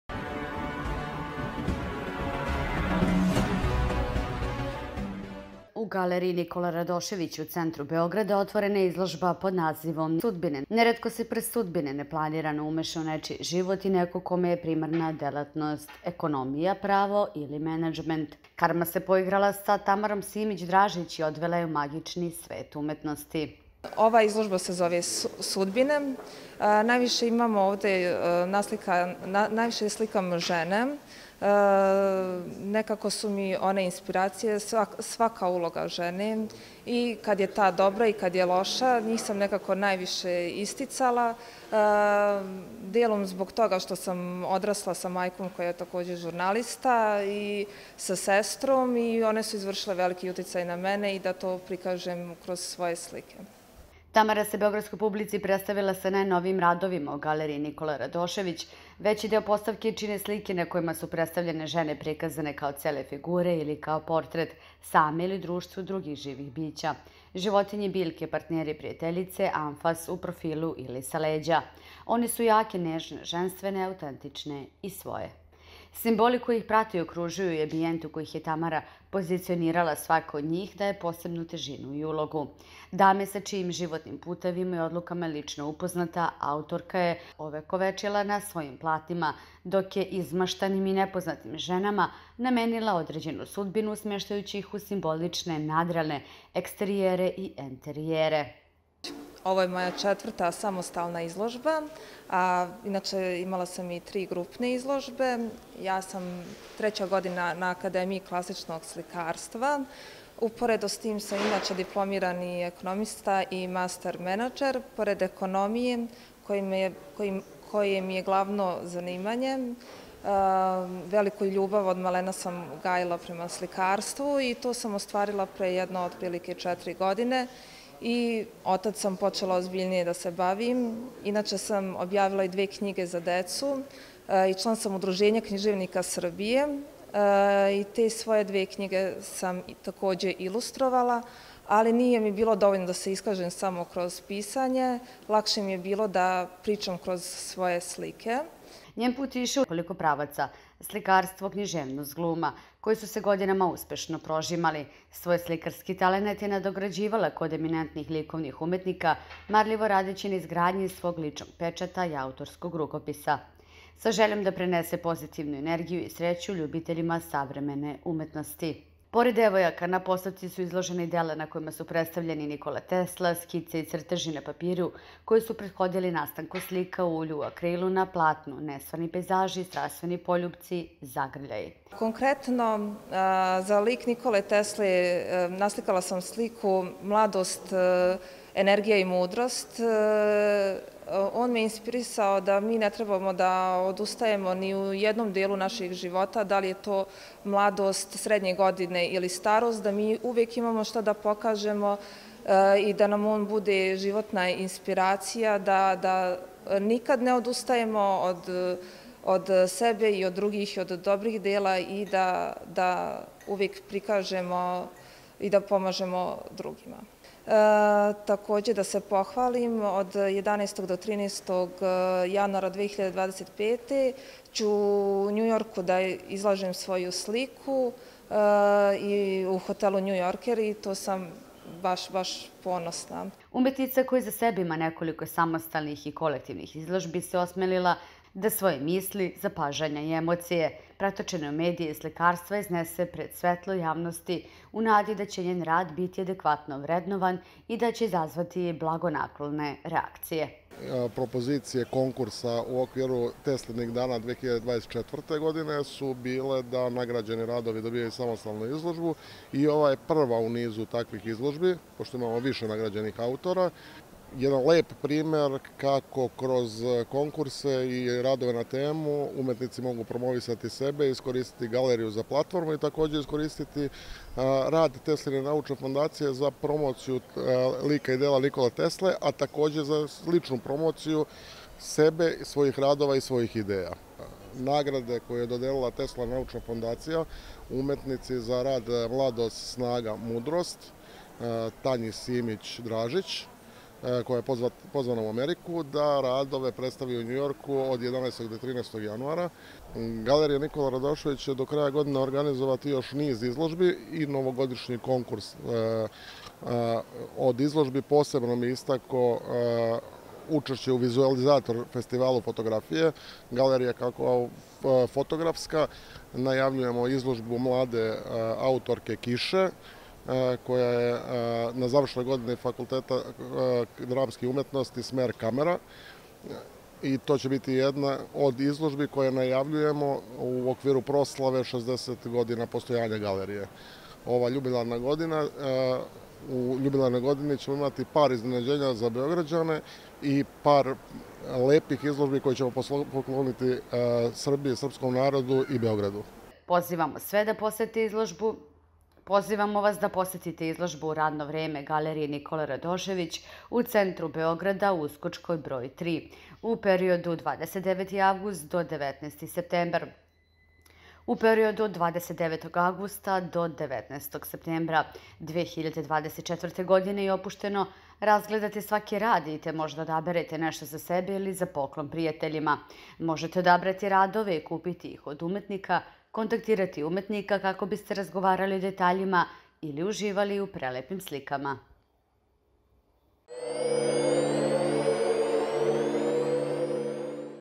U galeriji Nikola Radošević u Beogradu, Izložba pod nazivom. - SUDBINA -Tamare Simić Dražić.
U galeriji Nikola Radošević u Beogradu otvorena, Izložba pod nazivom.         SUDBINA -Tamare Simić Dražić

     U galeriji Nikola Radošević u centru Beograda otvorena je  Izložba pod nazivom  - SUDBINA -  Neretko se prst sudbine neplanirano umeša u nečiji život, i neko kome je primarna delatnost ekonomija, pravo ili
menadžment, odluta u nepregledno polje kreativnosti i
mašte. Karma se poigrala sa Tamarom Simić Dražić i
odvela je u magični svet umetnosti.
     Njen put je išao u nekoliko pravaca ( slikarstvo, književnost, gluma... ) koji su se godinama uspešno prožimali . Svoj slikarski talenat je
nadograđivala kod eminentnih likovnih umetnika, marljivo radeći na izgradnji svog ličnog pečata i autorskog rukopisa. Sa željom da prenese pozitivnu energiju i sreću ljubiteljima savremene umetnosti.
Tamara se beogradskoj publici predstavila sa najnovijim radovima u galeriji „ Nikola Radošević „. Veći deo postavke čine slike na kojima su predstavljene žene prikazane kao cele figure ili kao portret; same ili u društvu drugih živih bića ( životinje, biljke, partneri, prijateljice... ); anfas, u
profilu ili sa leđa. One su jake, nežne, ženstvene, autentične i svoje.
   Simboli koji ih prate i okružuju i ambijent u koji ih je Tamara pozicionirala svakoj od njih daje posebnu težinu i ulogu . Dame sa čijim životnim
putevima i odlukama je lično upoznata, autorka je ovekovečila na svojim platnima, dok je izmaštanim i nepoznatim ženama namenila određenu sudbinu smeštajući ih u simbolične, nadrealne eksterijere i
enterijere.
   Pored devojaka, na postavci su izložena i dela na kojima su predstavljeni : Nikola Tesla, skice i crteži na papiru koje su prethodili nastanku slika u ulju i akrilu na platnu, nestvarni pejzaži, strastveni poljupci , zagrljaji...
    Umetnica koja iza sebe ima nekoliko samostalnih i kolektivnih izložbi se osmelila da svoje misli, zapažanja i emocije pretočene u medij slikarstva iznese pred svetlo javnosti u nadi da će njen rad biti adekvatno vrednovan i
da će izazvati blagonaklone reakcije
    Pozivamo sve da posete izložbu u radno vrieme galerije Nikola Radošević u centru Beograda u Uskočkoj broj -3.
– U periodu  od: 2 9 . avgusta  -do - 1 9 . septembra. -  2 0 2 4. godine,   i opušteno razgledaju svaki rad te, možda, odaberu nešto za sebe ili za poklon prijateljima. možete odabrati radove i kupiti ih  od umetnika, kontaktirati umetnika kako biste razgovarali o detaljima ili uživali u  prelepim slikama.